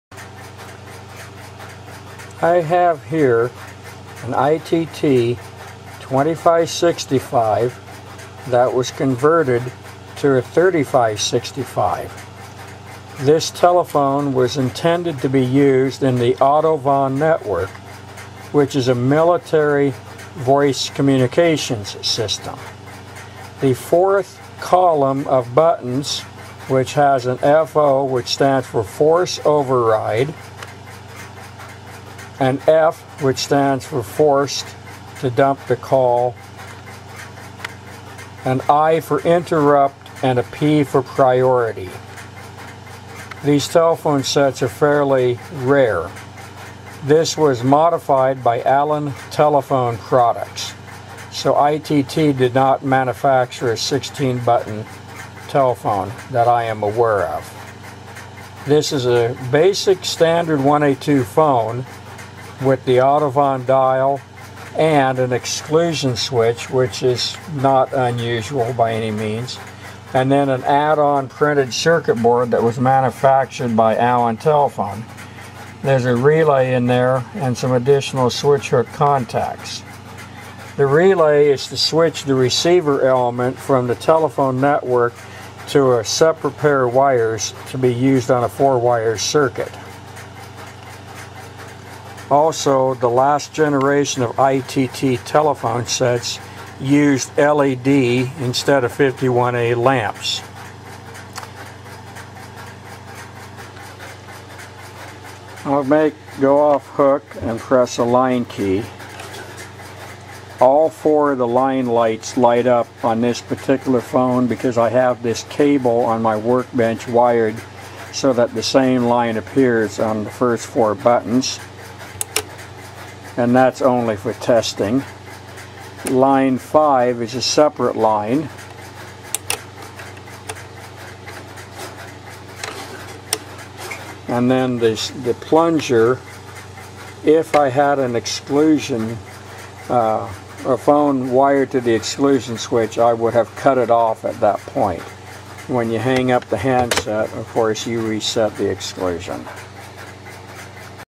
1A2 Autovon phone
Here is a converted ITT 2656 1A2 6 button to Allen tel 3568 Autovon Phone. I made 2 errors in the narration. Will fix later.